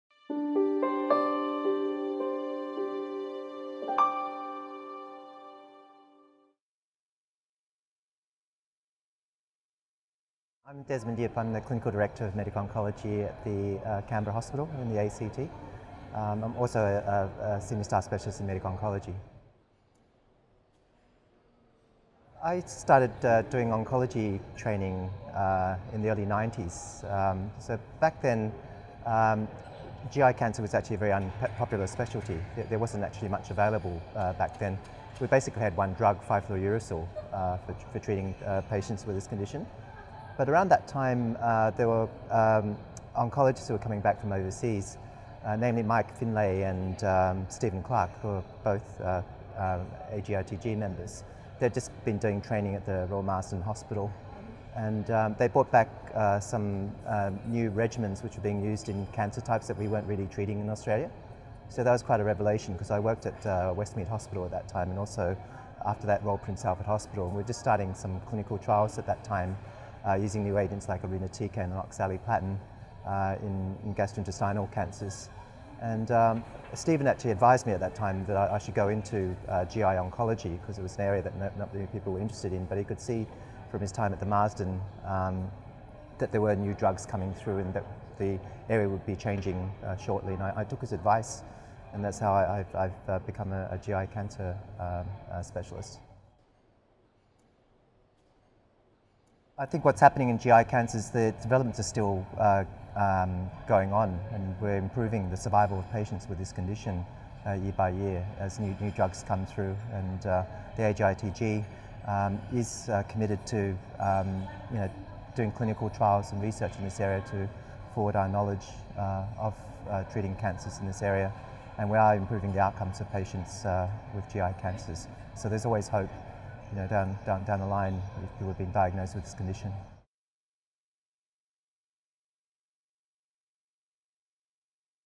Professor Desmond Yip - Why I became an oncologist
Professor Desmond Yip speaks about why he became involved in gastro-intestinal cancer research.